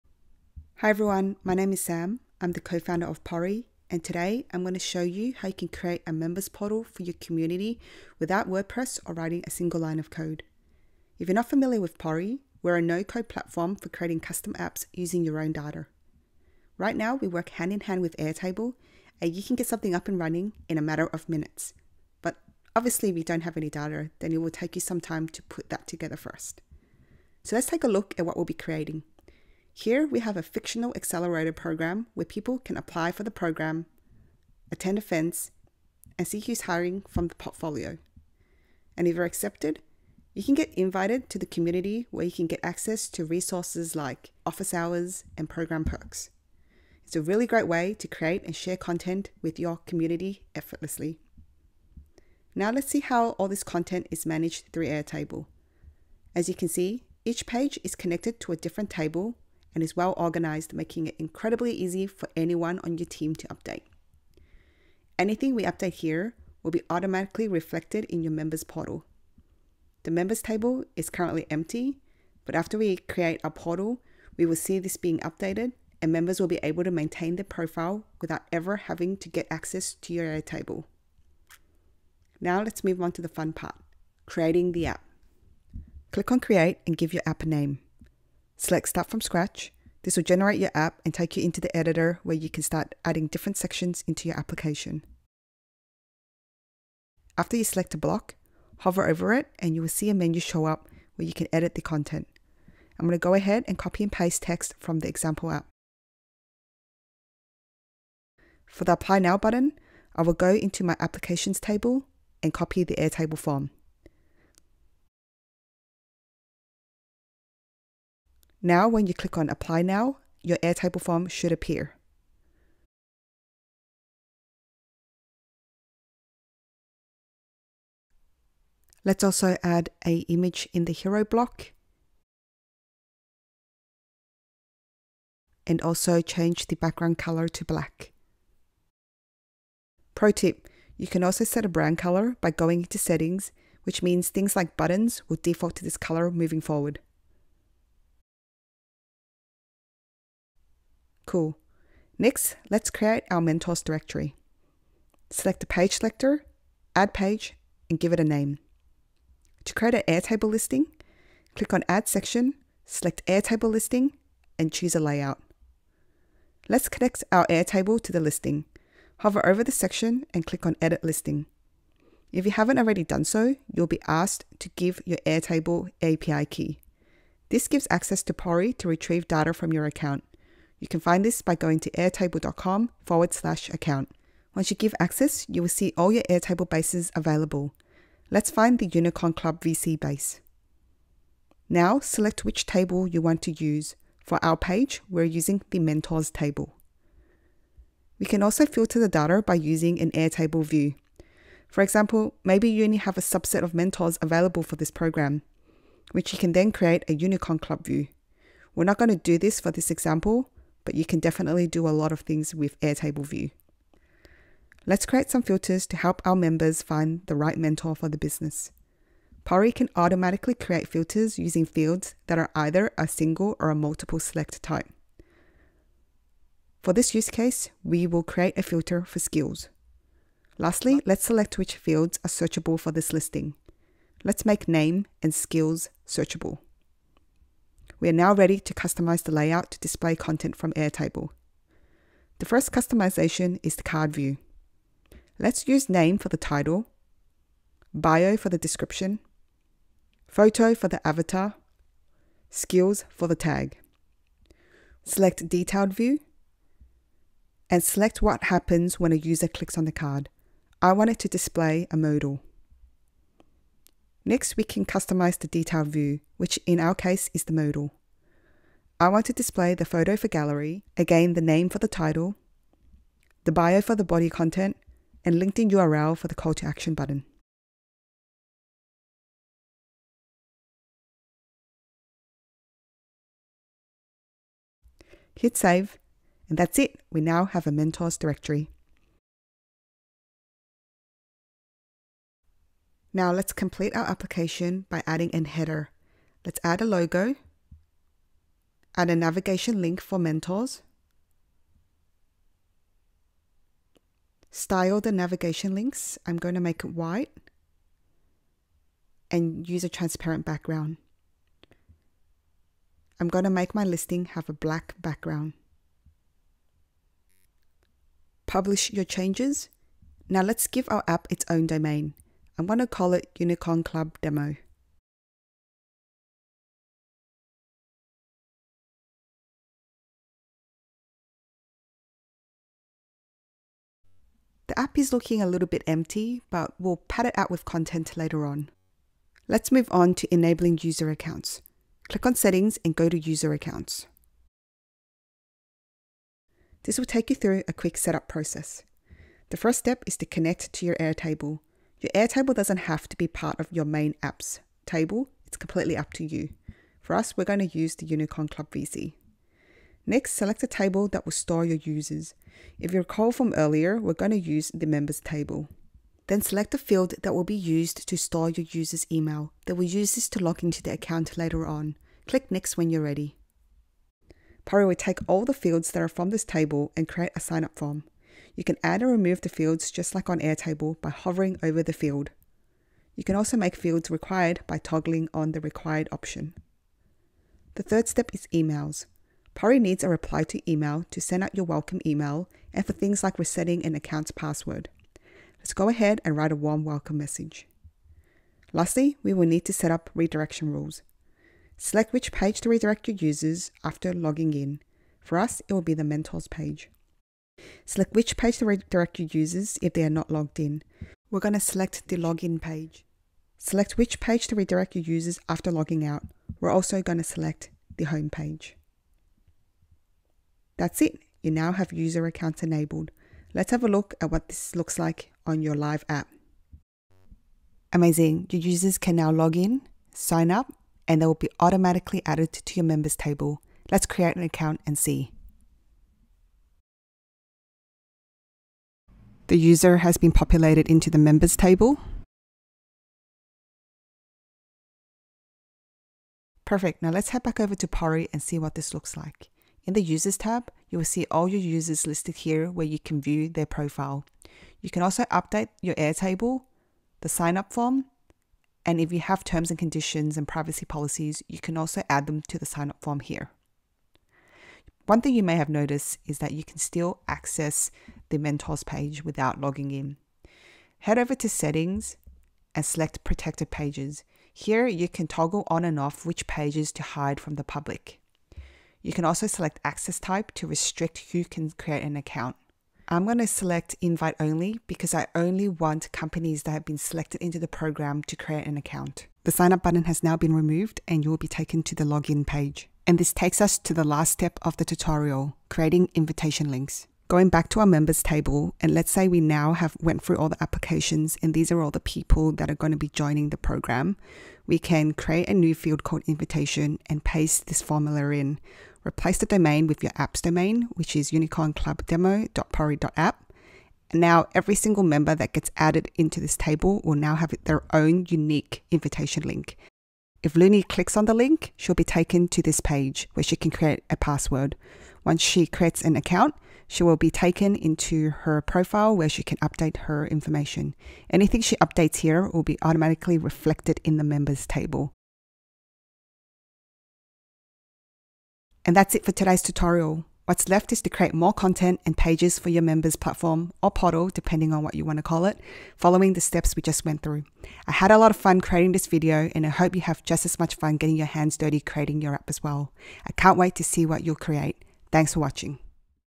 Build a members directory with Airtable [Wordpress Alternative]
Hey everyone! In this video I'm going to show you how to can create a members portal for your community.

The tutorial will cover:

00:00:00 Introduction to Pory
00:00:52 Using Airtable as a CMS
00:01:22 Building the App on Pory
00:05:39 Setting up user account
00:07:44 Protecting pages from non-members
00:08:12 Automatically creating invitation links

All content is synced with your Airtable, including member profiles so you'll never have to manually update data again!